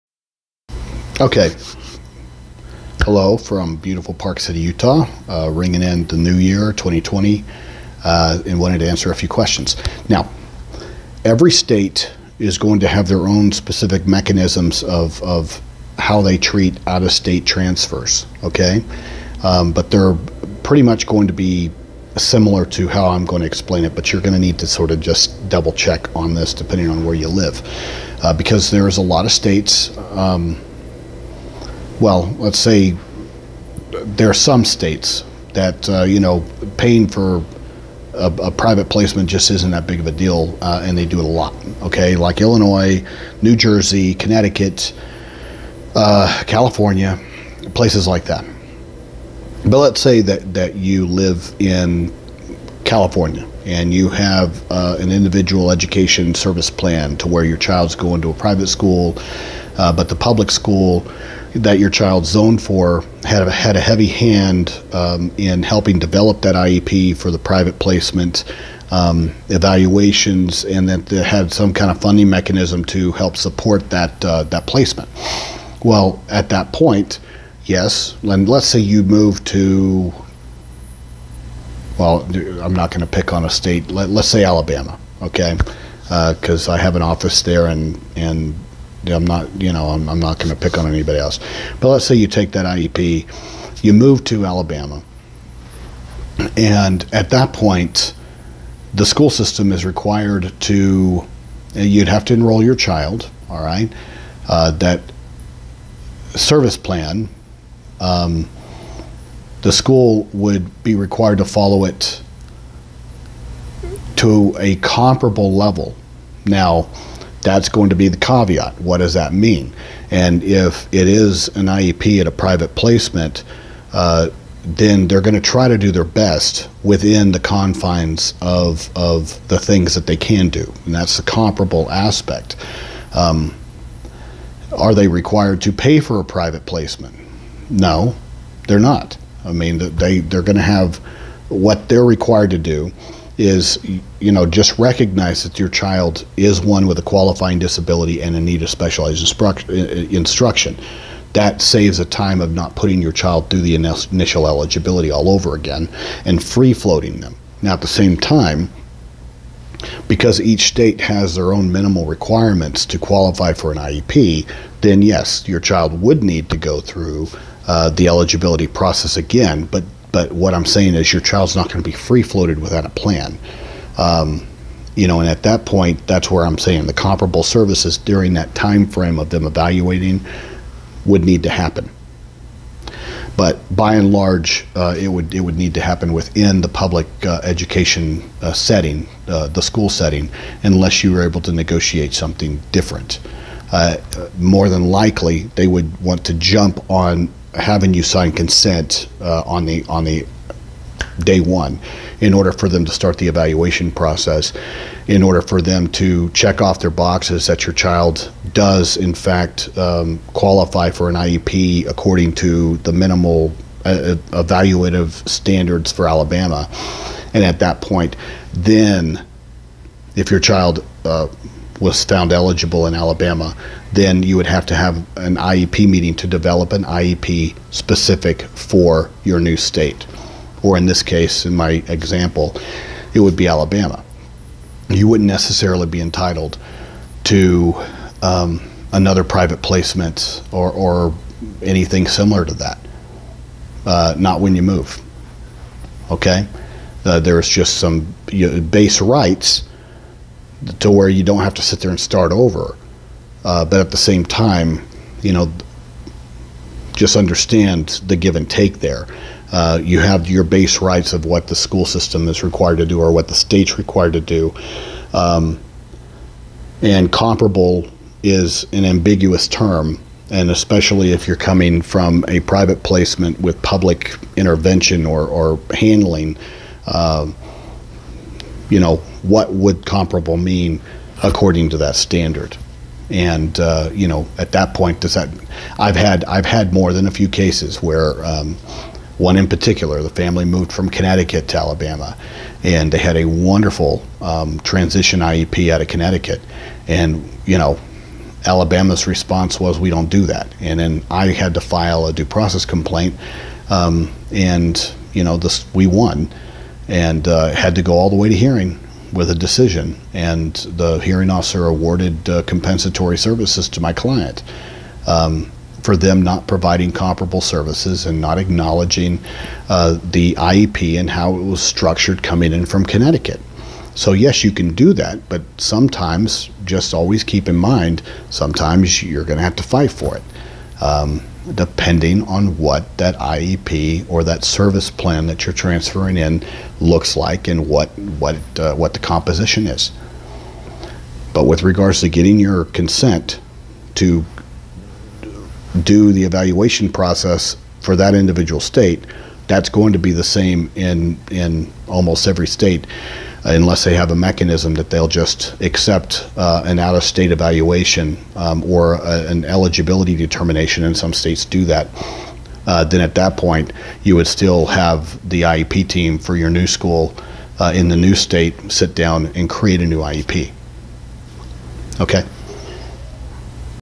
IEP or IESP and what happens when you transfer to another state. Will the IEP or IESP be recognized?
The Question Asked:
"If a child has an IESP (S for Service because the child was enrolled in a private school), and transfers states, does the new state have to accept that IESP as an IEP and honor it? The public school system provided the evaluations, services and was part of the team to create the IESP"